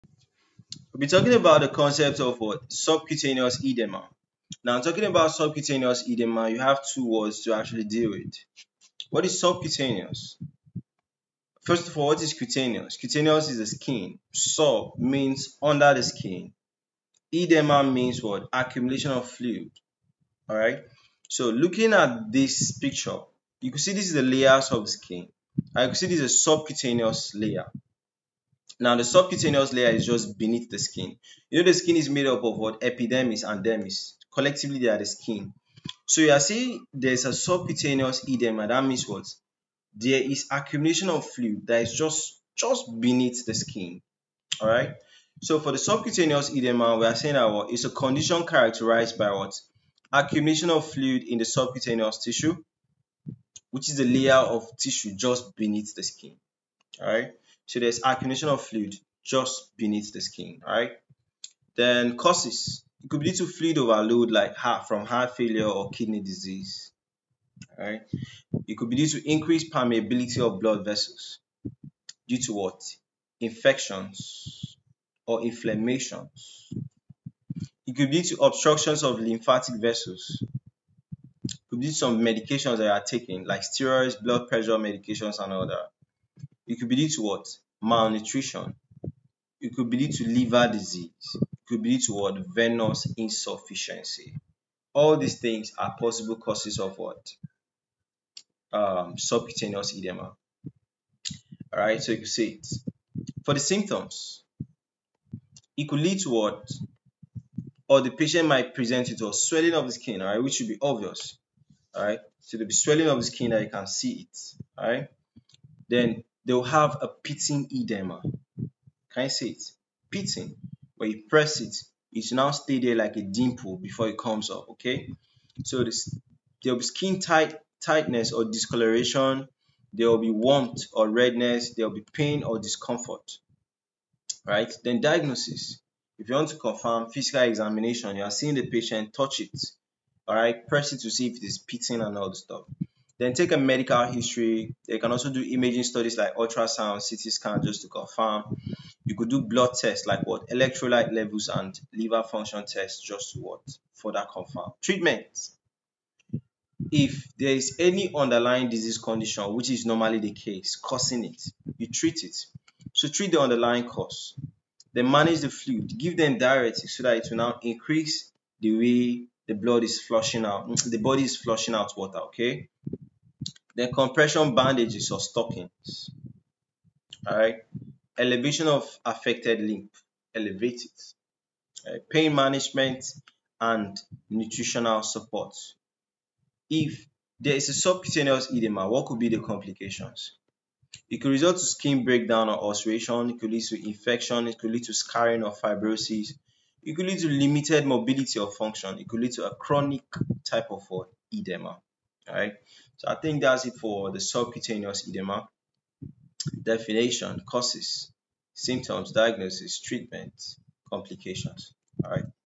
Subcutaneous Edema ; Definition, Causes, Symptoms, Diagnosis, Treatment, Complications
Subcutaneous edema, also known as subcutaneous swelling, occurs when excess fluid accumulates in the subcutaneous tissue, which is the layer of tissue just beneath the skin.

Causes:

1. Fluid retention due to heart failure, kidney disease, or liver disease
2. Inflammation or infection
3. Trauma or injury
4. Poor circulation or venous insufficiency
5. Medications, such as steroids or blood pressure medications
6. Allergic reactions

Symptoms:

1. Swelling or puffiness in the affected area
2. Skin may feel tight or stretched
3. Discomfort or pain
4. Skin may be warm or tender to the touch
5. Reduced mobility or flexibility

Common areas affected:

1. Legs and ankles
2. Arms and hands
3. Face and eyelids
4. Abdomen and genital area

Treatment:

1. Address underlying cause
2. Compression bandages or stockings
3. Elevation of affected area
4. Diuretics to reduce fluid retention
5. Pain management with medication
6. Antibiotics if infection is present

It's essential to consult a healthcare professional for proper diagnosis and treatment to manage subcutaneous edema effectively.